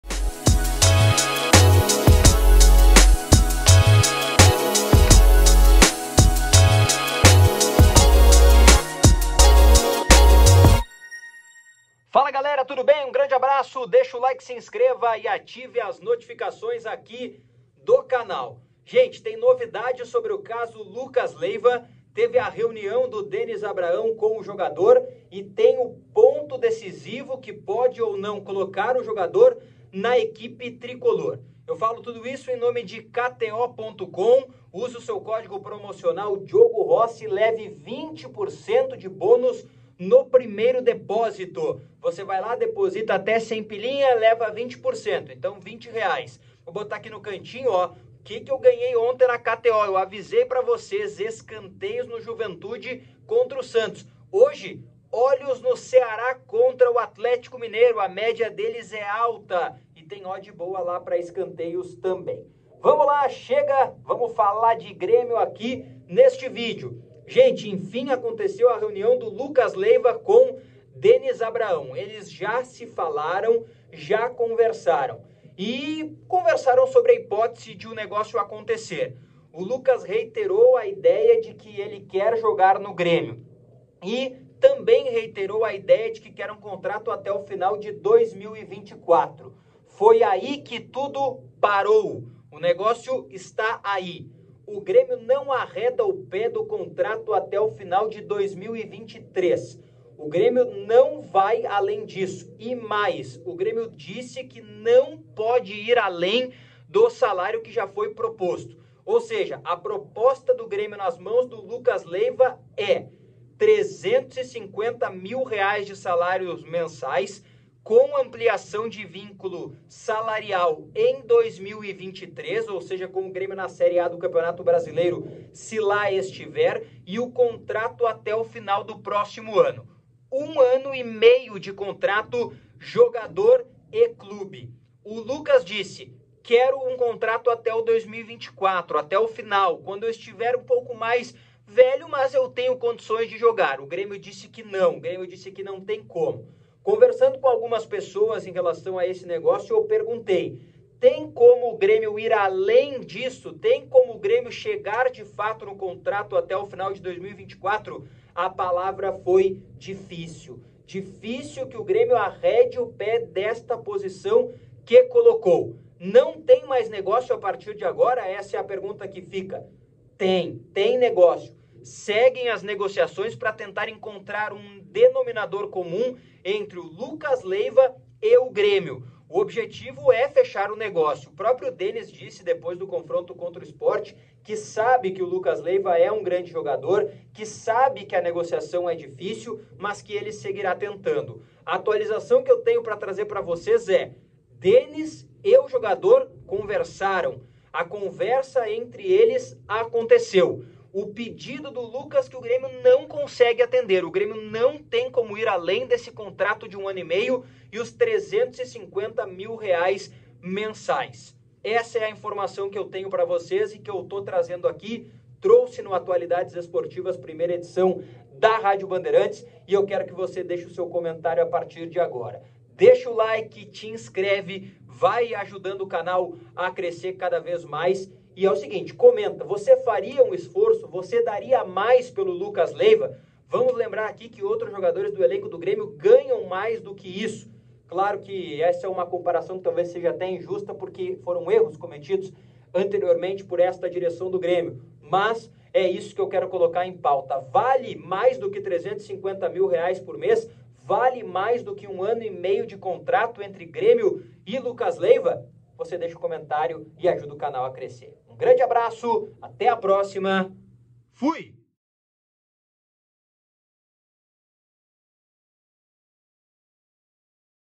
🚨 GRÊMIO: DETALHES DA CONVERSA ENTRE DENIS ABRAHÃO E LUCAS LEIVA! | DIVERGÊNCIA ENTRE AS PARTES!
Detalhes da reunião entre Grêmio e Lucas Leiva!

Chegamos na parceria da KTO.com!